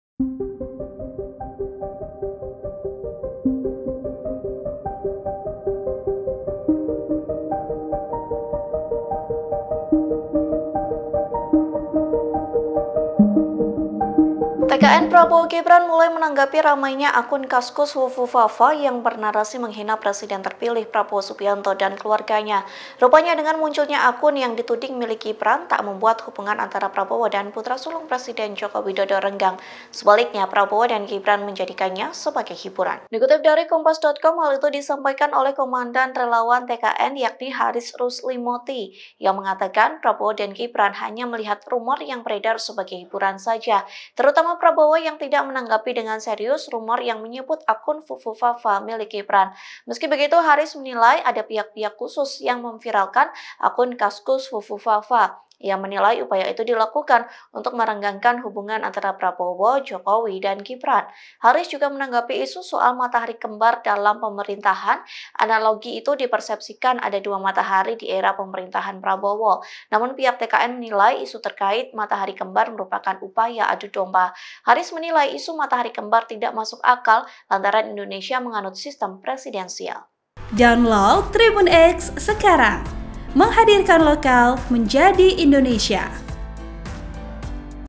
Prabowo 'Terkekeh' soal Akun Fufufafa Bernarasi Menghinanya, TKN: Jadi Hiburan Prabowo dan Gibran
TRIBUN-VIDEO.COM - TKN Prabowo-Gibran mulai menanggapi ramainya akun Kaskus fufufafa yang bernarasi menghina Presiden Terpilih Prabowo Subianto dan keluarganya.

Rupanya dengan munculnya akun yang dituding milik Gibran, tak membuat hubungan antara Prabowo dan putra sulung Presiden Joko Widodo renggang.

Sebaliknya, Prabowo dan Gibran menjadikannya sebagai hiburan.

Dikutip dari Kompas.com, hal itu disampaikan oleh Komandan Relawan TKN, Haris Rusly Moti.

Ia mengatakan Prabowo dan Gibran hanya melihat rumor yang beredar sebagai hiburan saja.

Terutama Prabowo tidak menanggapi dengan serius rumor yang menyebut akun fufufafa milik Gibran.

“Baik Pak Prabowo maupun Mas Gibran menilai rumor seperti ini sebagai hiburan yang tidak perlu disekapi serius,” kata dia.

Meski begitu, Haris menilai ada pihak-pihak khusus yang memviralkan akun Kaskus fufufafa.

Ia menilai upaya itu dilakukan untuk merenggangkan hubungan Prabowo, Jokowi, dan Gibran.

Haris juga menanggapi isu soal matahari kembar dalam pemerintahan.

Analogi itu dipersepsikan ada dua matahari di era pemerintahan Prabowo.

Namun pihak TKN menilai isu terkait matahari kembar merupakan upaya adu domba.

Haris menilai isu matahari kembar tidak masuk akal lantaran Indonesia menganut sistem presidensial.

(Tribun-Video.com)

Artikel ini telah tayang di Kompas.com dengan judul "Heboh Akun Fufufafa, TKN Tuding Ada yang Adu Domba Prabowo dengan Jokowi-Gibran", Klik untuk baca

Program: Tribunnews Update
Host: Maria Nanda Ayu Saputri
Editor Video: Ni'amu Shoim Assari Alfani
Uploader: Bintang Nur Rahman